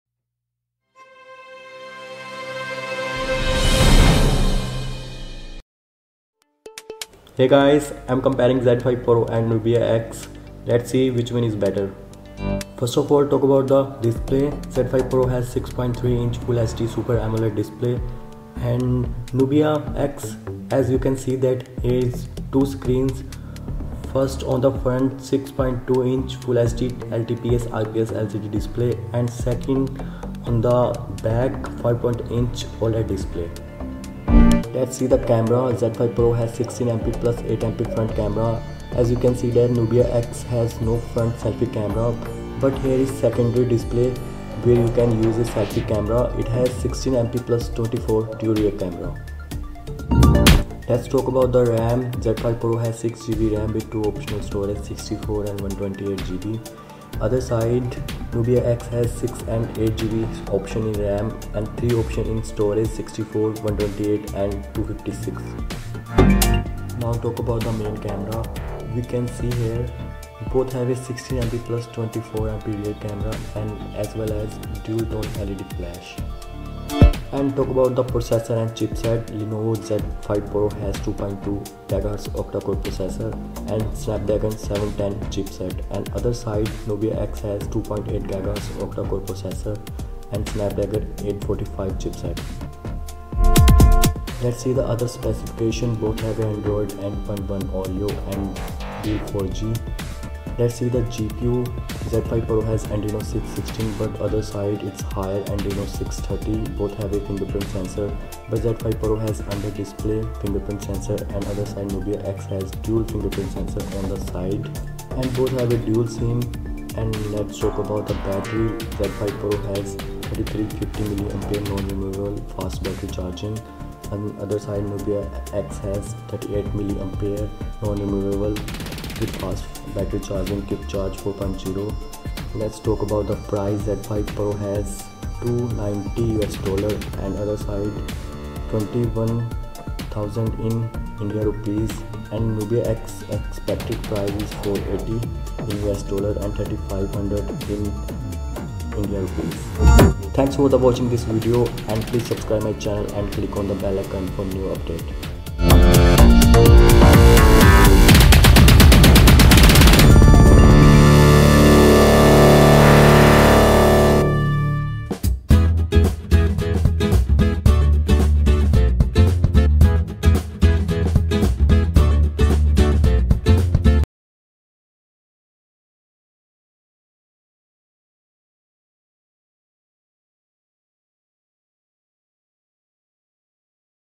Lenovo Z5 Pro Vs ZTE Nubia X Specs, Price, Design Comparison
Lenovo Z5 Pro Vs ZTE Nubia X Comparison